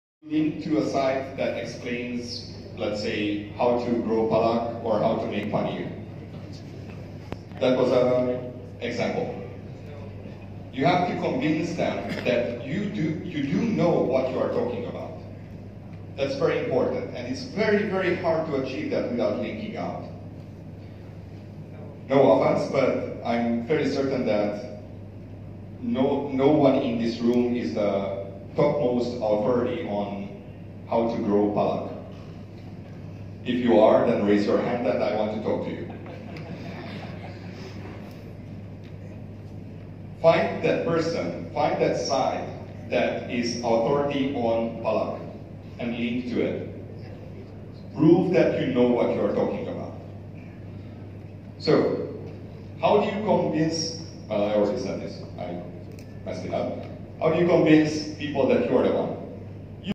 Linking Out Most Vital For SEO -Gary Explained Details at Webmaster Conference - Google
Gary Google Search Team Head Describe about latest SEO Tricks at
Kolkata Webmaster Conference